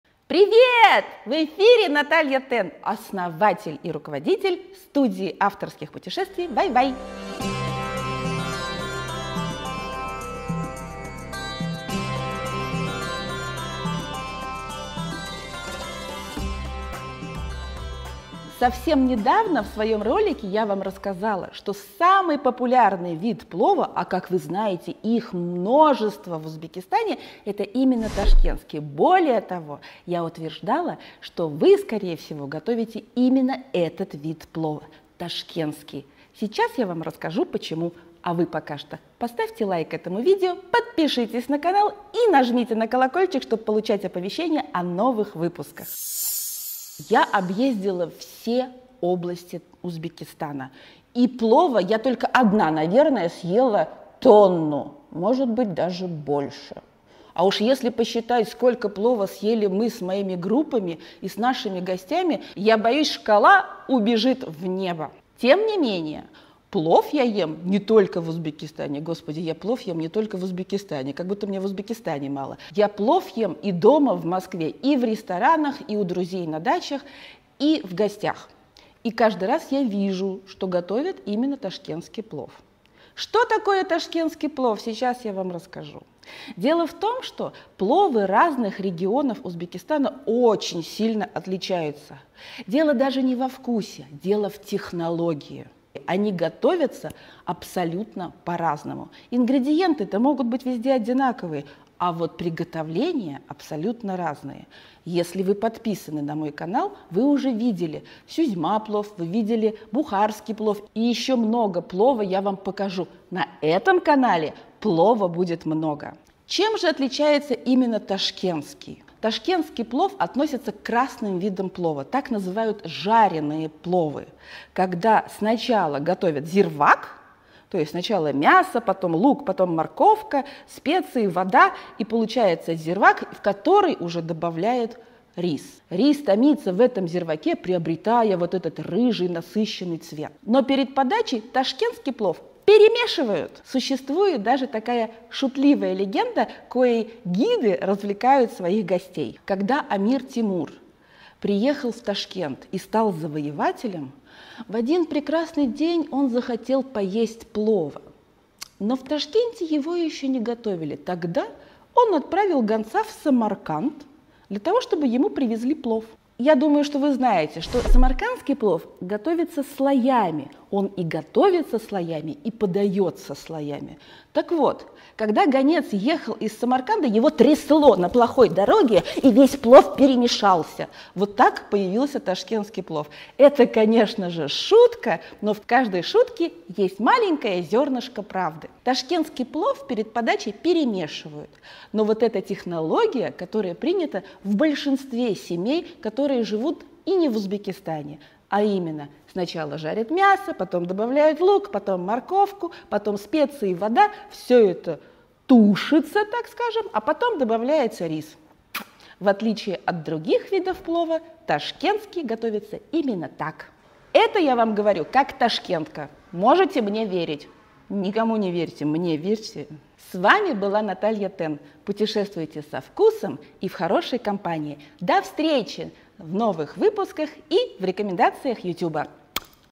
ТАКОЙ ПЛОВ ГОТОВЯТ ЧАЩЕ ВСЕГО!
В Узбекистане каждый регион может похвастаться своим видом плова, но есть вид плова (а точнее - технология приготовления), которая распространилась очень широко. Многие мои друзья и знакомые готовят плов таким образом дома и на даче. Таким пловом вас накормят в ресторане. И я уверена, что мои зрители чаще всего готовят именно этот плов - ташкентский.
В чём его особенность и как он появился (по легенде) - рассказываю в новом выпуске.
Наверняка у вас будут дополнения и замечания, пишите их в комментариях. Плов - это отличный вкусный повод для обсуждения!

эпизоды:
00:00 начало
00:18 самый популярный вид плова
01:25 что такое ташкентский плов
02:23 шутливая легенда
03:40 заключительная часть